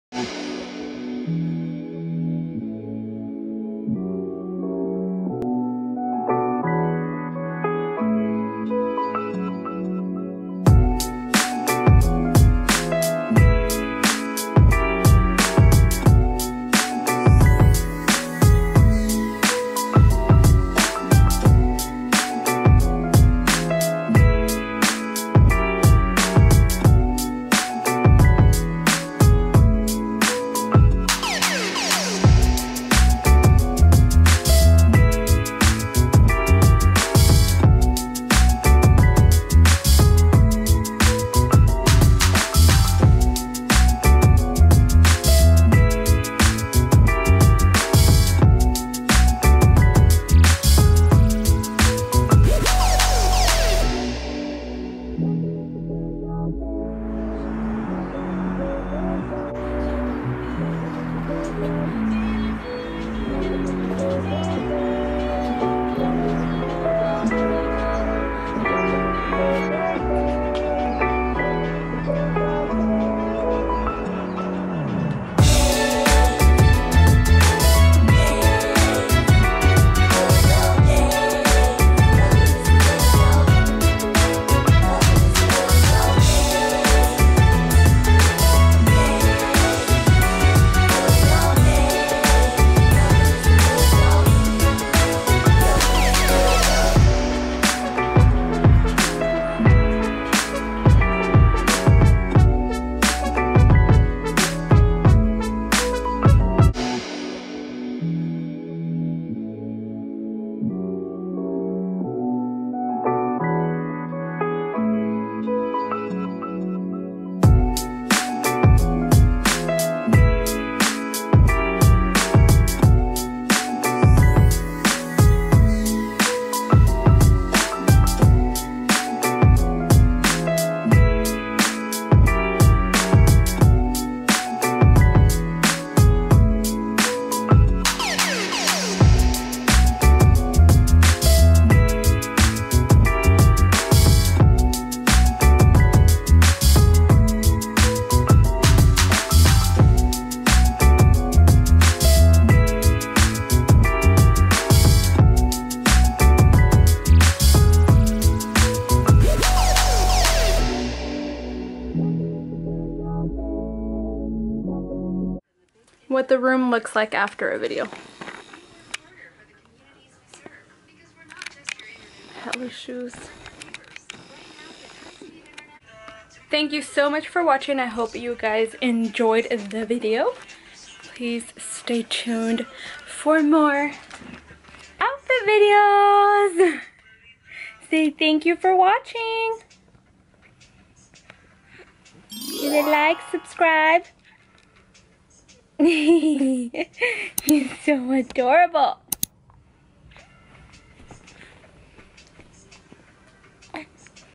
2 PIECES 3 DIFFERENT STYLES
Hope you guys enjoyed this video I will be posting more fashion related videos soon.  Let me know what piece of clothing you want me to style next. What outfit was your favorite? Thank you so much.